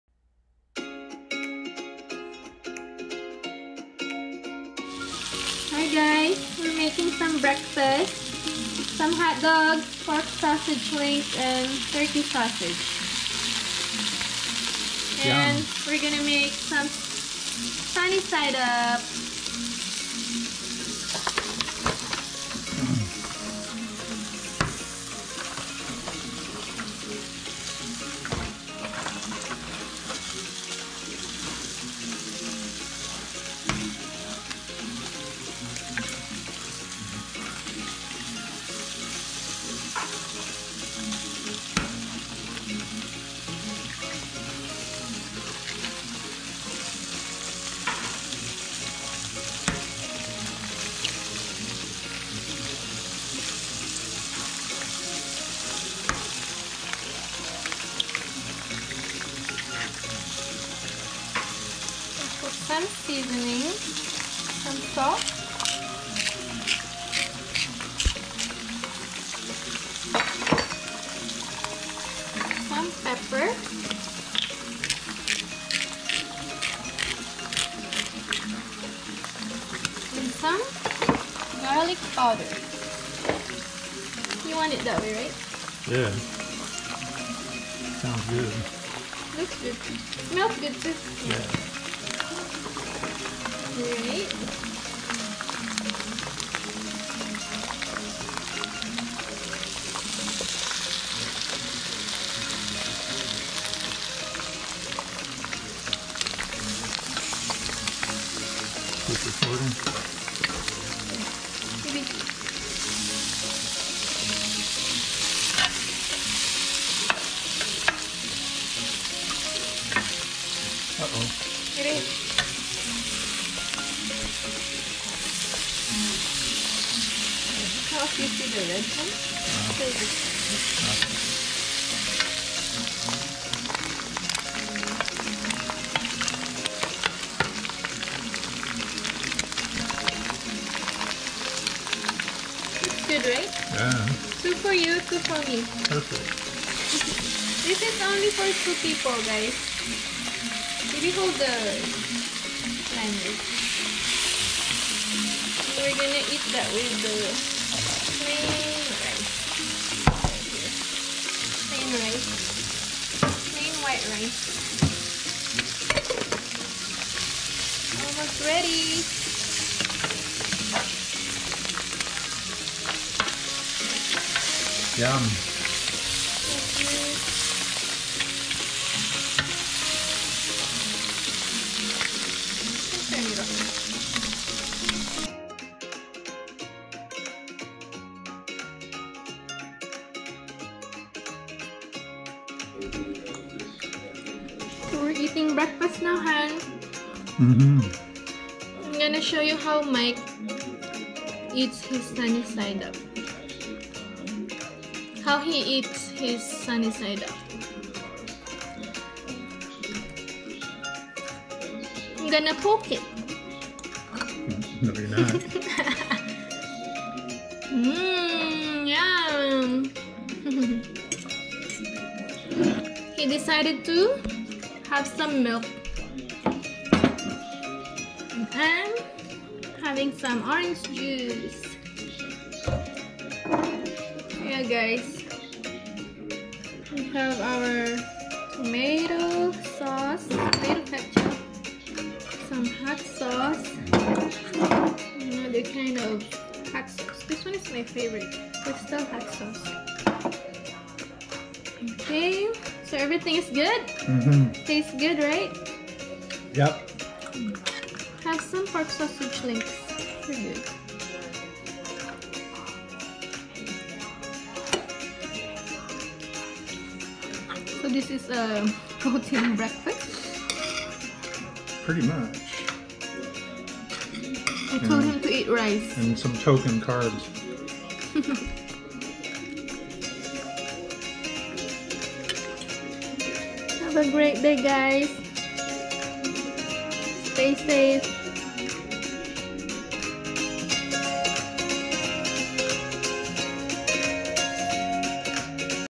BREAKFAST!
Made us some breakfast this morning. It was very tasty.